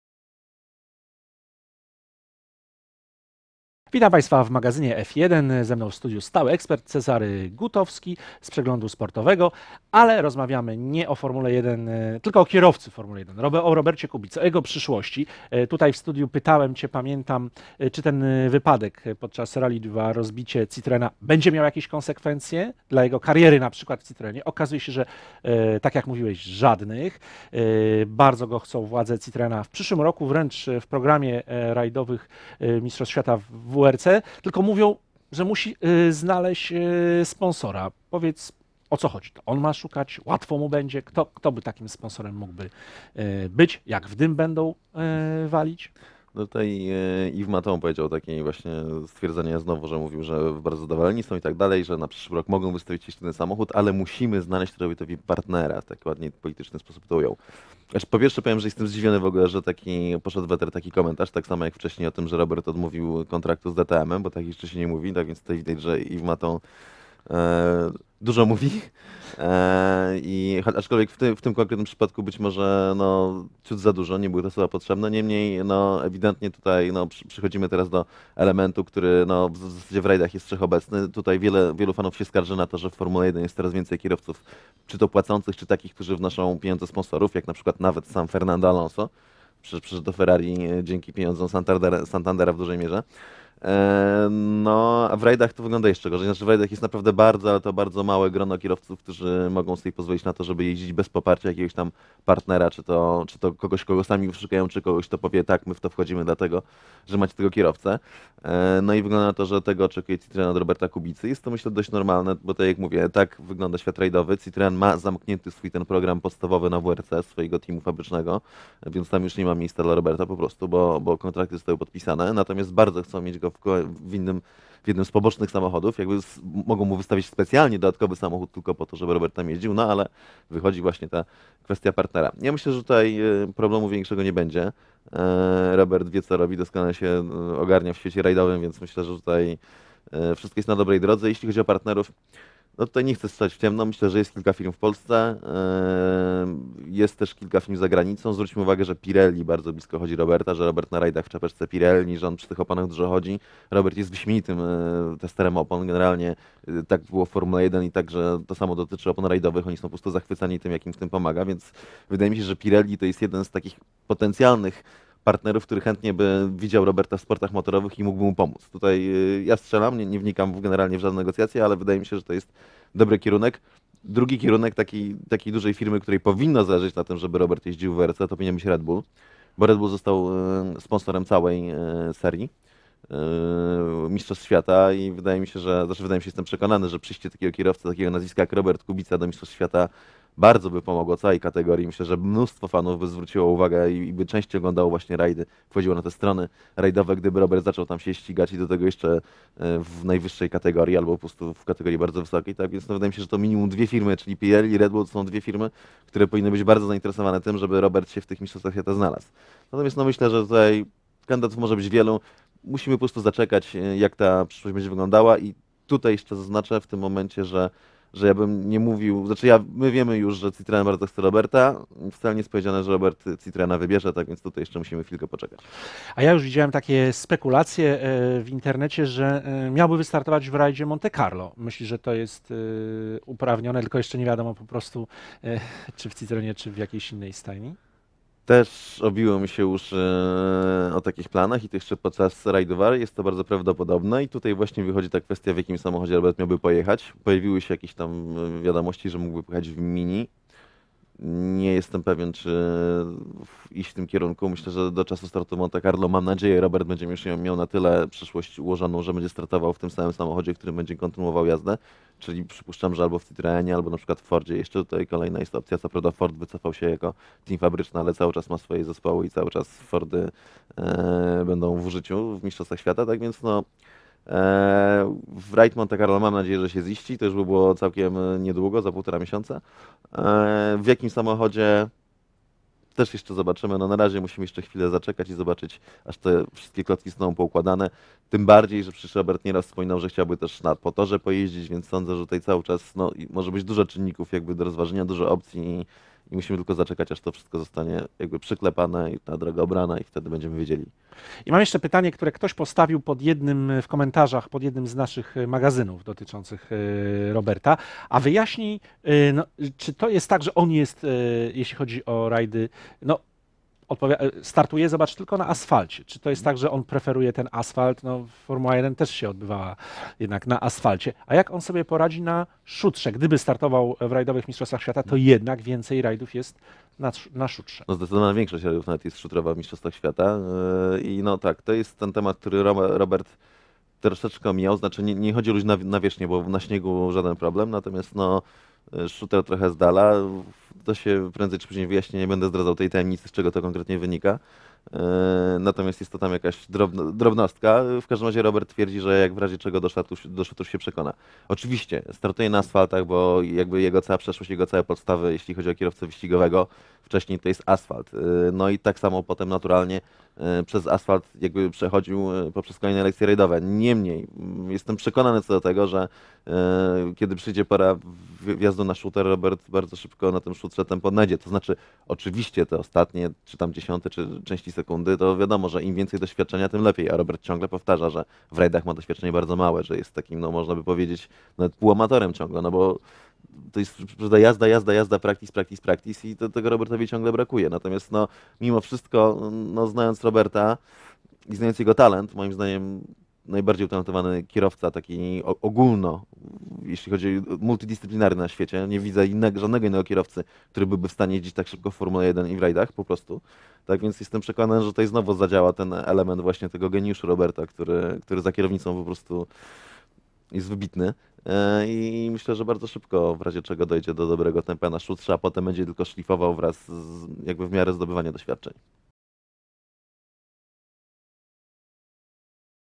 Kubica bez problemu znajdzie sponsorów na rajdowe MŚ? Jak poradzi sobie na szutrze?
Sport.pl prezentuje: Krystalizuje się przyszłość Roberta Kubicy na przyszły sezon. Szef rajdowego zespołu Citroena, Yves Matton publicznie stwierdził, że jego teama bardzo chce by Polak startował w mistrzostwach świata, musi tylko znaleźć sponsora. - Tak wygląda świat sportów motorowych. Wnoszenie przez zawodników pieniędzy do teamu nie jest niczym nowym, nawet Fernando Alonso trafił do Ferrari przy wsparciu banku Santander. Citroen ma już zamknięty swój program fabryczny na WRC, kontrakty zostały podpisane. Bardzo chcą wystawić też samochód Roberowi, muszą tylko znaleźć partnera. Większych problemów nie będzie. Zwróćmy uwagę na jego współpracę z Pirelli, które wie, że Polak jest świetnym testerem opon. Szczególnie zainteresowany powinien być też Red Bull, któremu jako sponsorowi całej serii, bardzo by pomogła jazda w rajdach takiej postaci jak Kubica - mówi w Magazynie F1 Cezary Gutowski z Przeglądu Sportowego. I zastanawia się czy Kubica wystartuje w Rajdzie Monte Carlo w... Mini i jak poradzi sobie w rajdach szutrowych, skoro startuje wyłącznie na asfalcie. Więcej wideo na